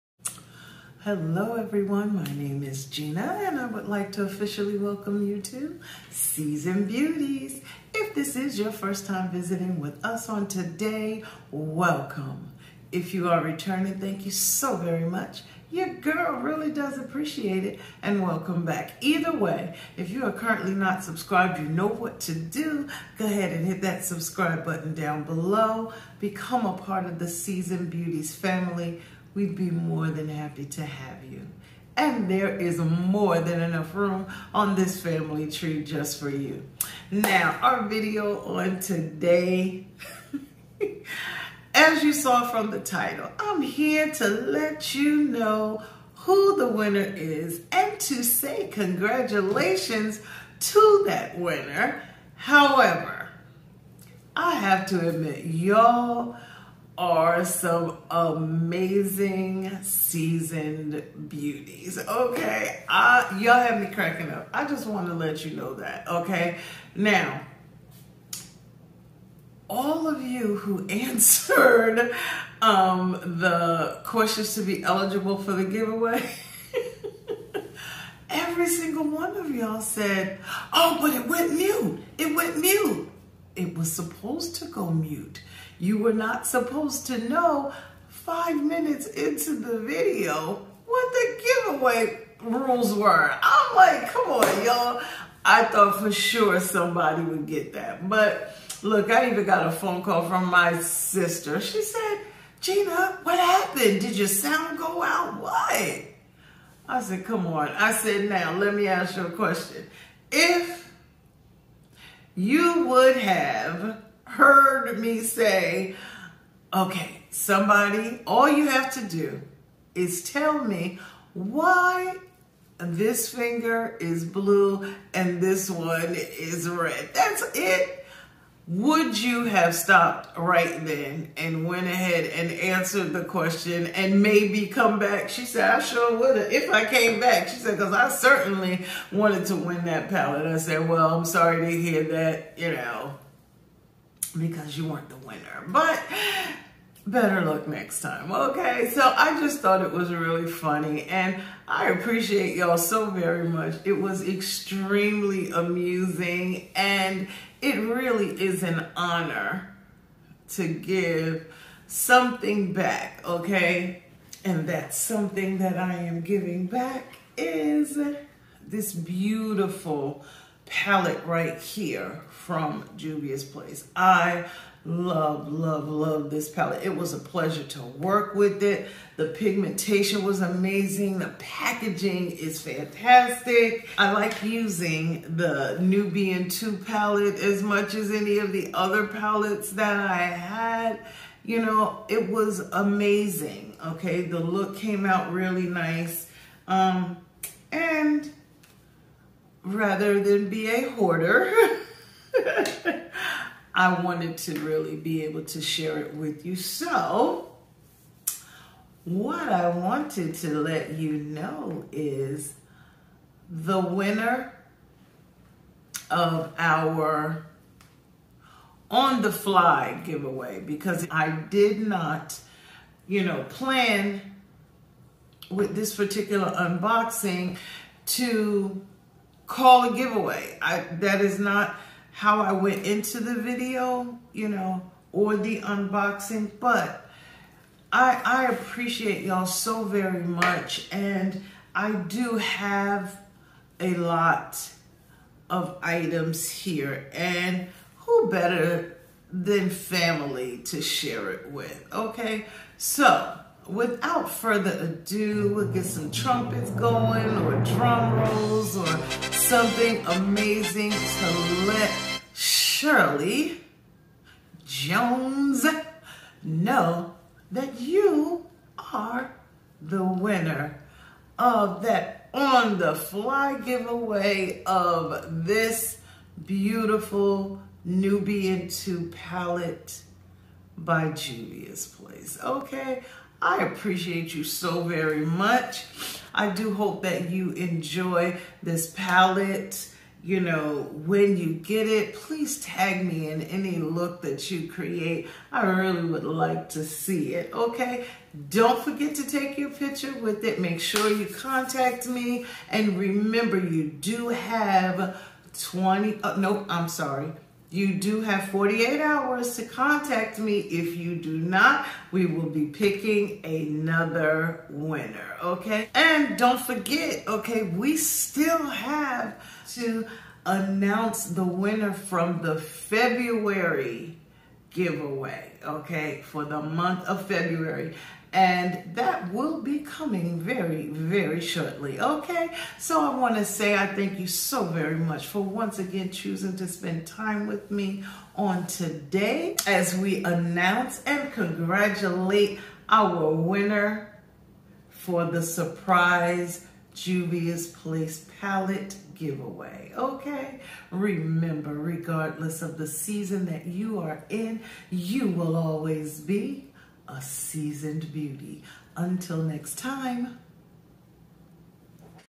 And the WINNER of the Surprise NUBIAN 2 pallet IS...
Today we announce the winner of the Suprise JP Nubian 2 giveaway.

beauty subscription boxes